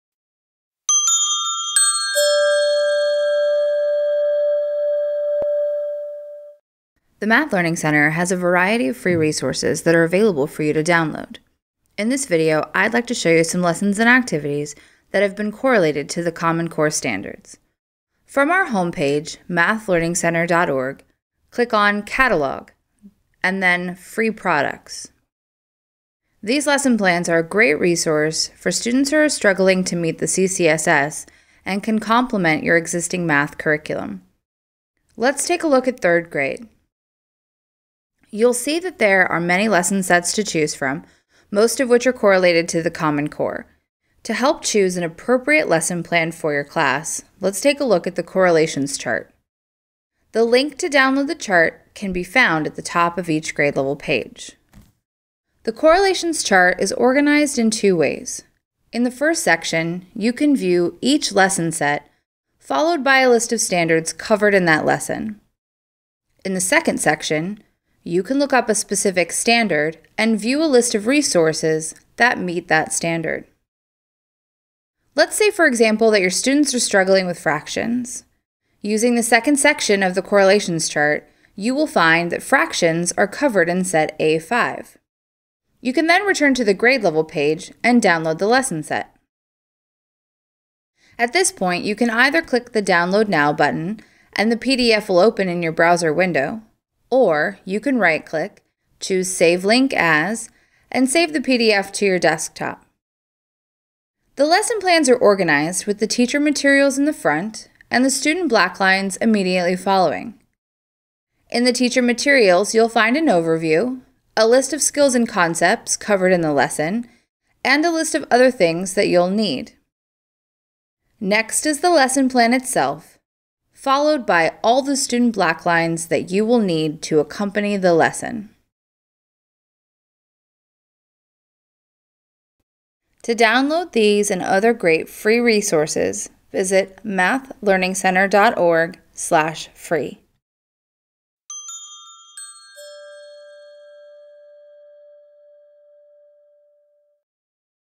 CCSS Lessons and Activities
Free downloadable lessons and activities that have been correlated to the Common Core State Standards.

For these and other great free resources,